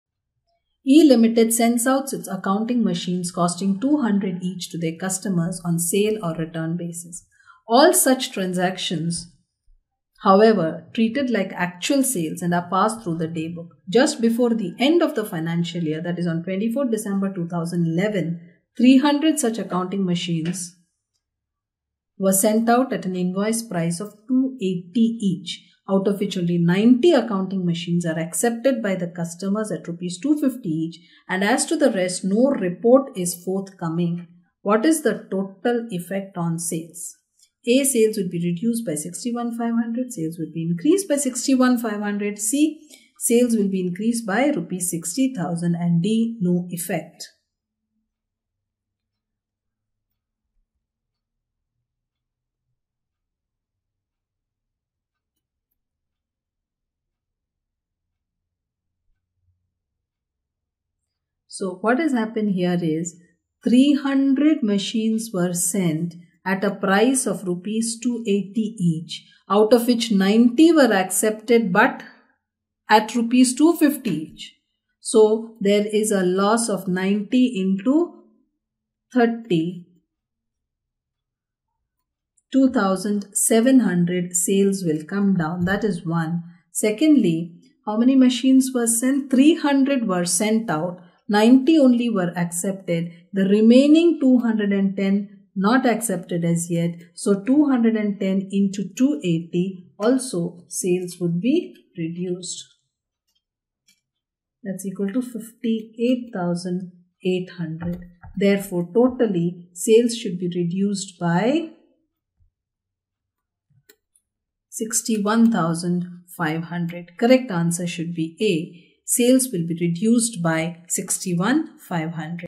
Additional Questions For Sale of Goods on Return Basis | Sale of Goods on Return Basis Accounting
Additional Questions For Sale of Goods on Return Basis, Learn Sales of Goods on Return Basis. We have covered What is Sales Return and Accoutning Treatment of Sales Return.For Details